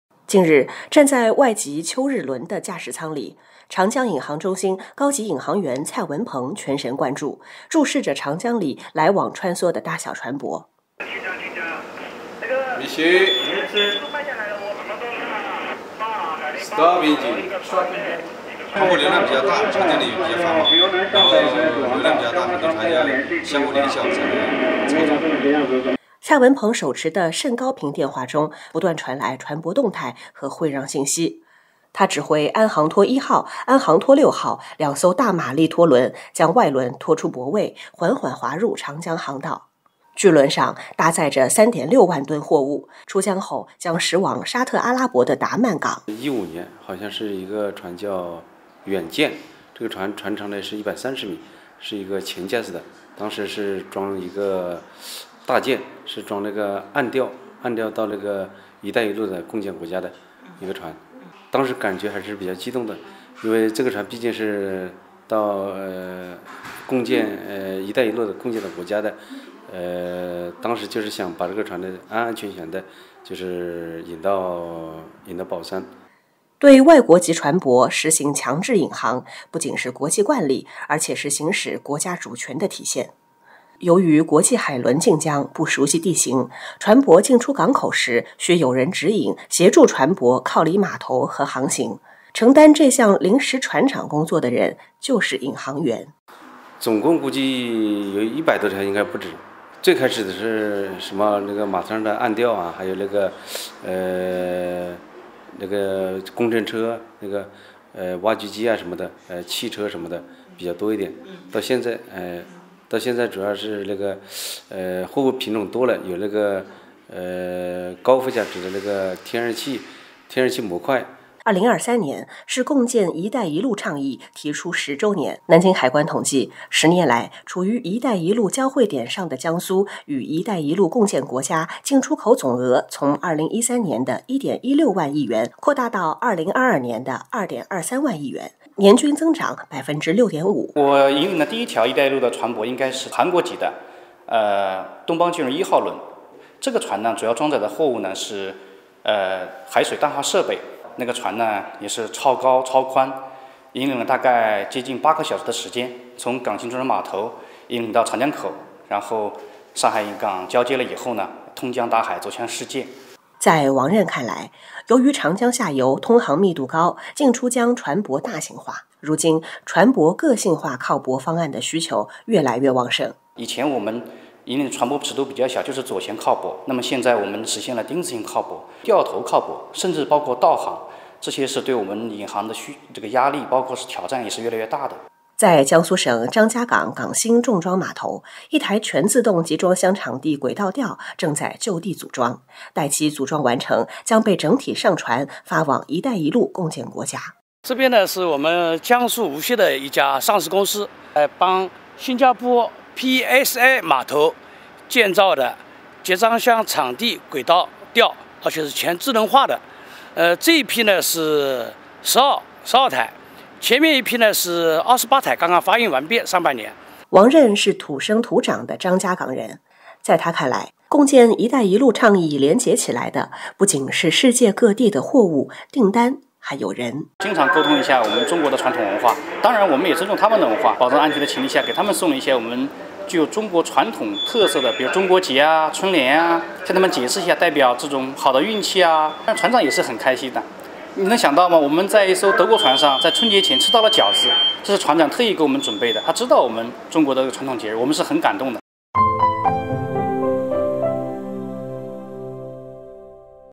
探访长江引航：引领“远洋驼队” 为“一带一路”方向船舶引航“出货”
近日，站在外籍“秋日”轮的驾驶舱里，长江引航中心高级引航员蔡文鹏全神贯注，注视着长江里来往穿梭的大小船舶。

对外国籍船舶实行强制引航，不仅是国际惯例，而且是行使国家主权的体现。由于国际海轮进江不熟悉“地形”，船舶进出港口时需有人指引、协助船舶靠离码头和航行。承担这项“临时船长”工作的人，就是引航员。

2023年是共建“一带一路”倡议提出十周年。南京海关统计，十年来，处于“一带一路”交汇点上的江苏与“一带一路”共建国家进出口总额从2013年的1.16万亿元扩大到2022年的2.23万亿元，年均增长6.5%。